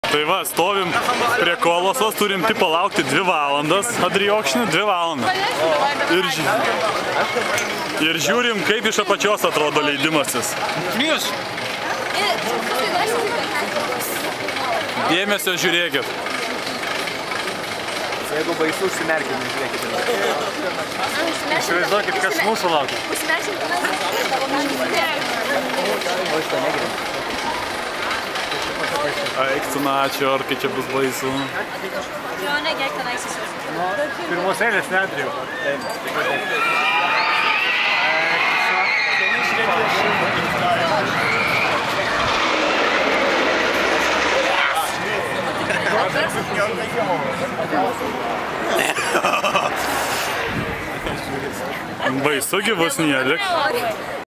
Heide Park Colossos - laukiam eilėje
Heidės parke stovim didžiulėje eilėje, kad galėtumėm pralėkt su aukščiausiu mediniu roller coasteriu Europoje